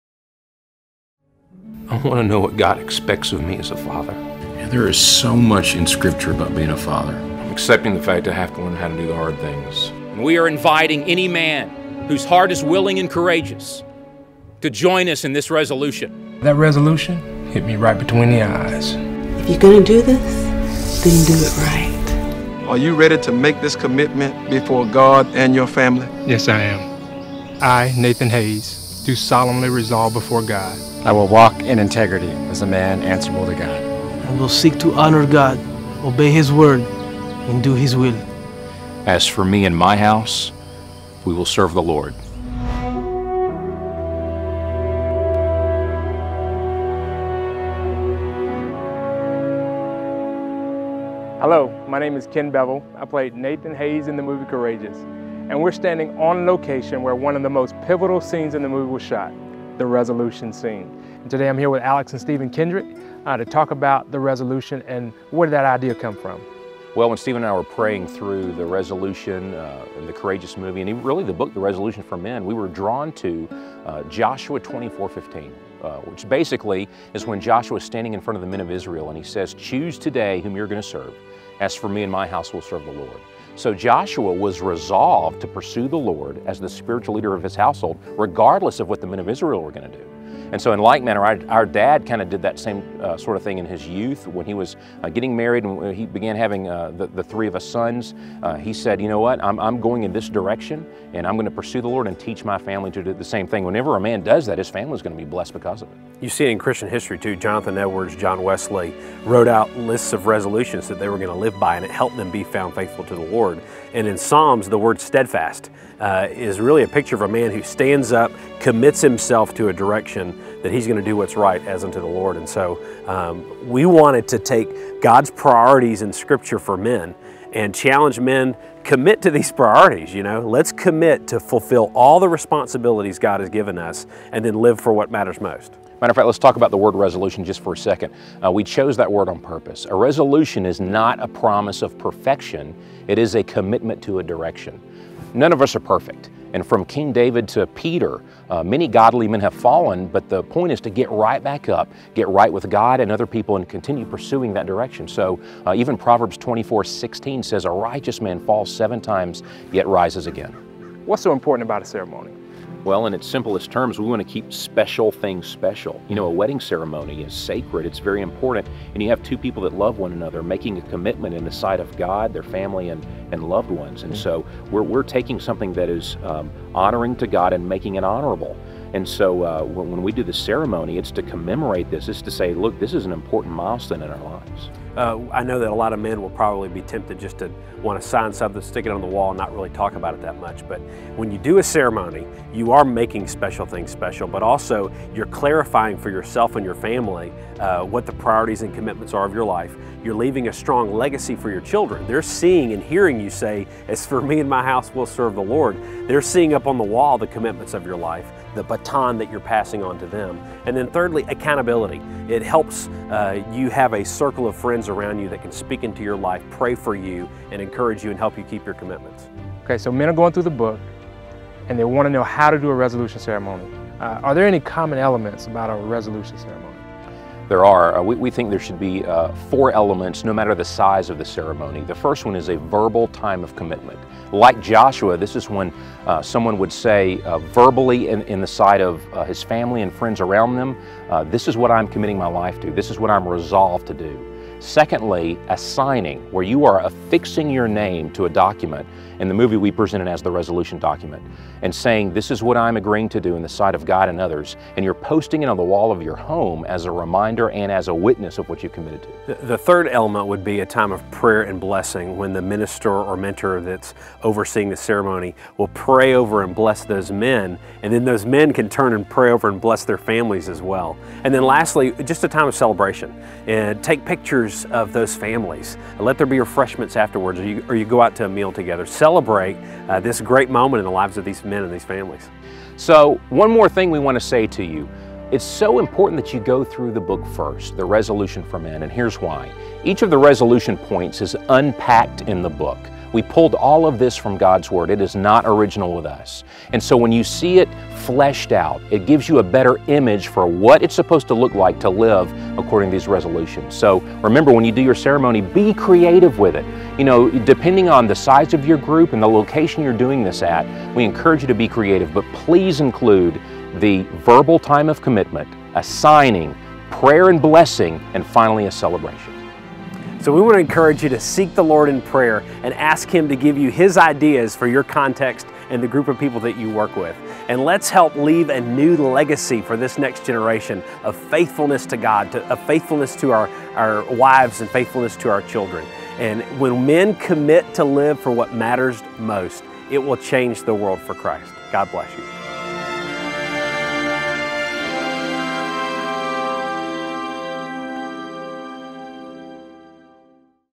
Host a Courageous Resolution Ceremony
Join host Ken Bevel, and Alex and Stephen Kendrick to gain insights on how you can participate in a powerful Courageous Resolution Ceremony to celebrate your commitment to your family.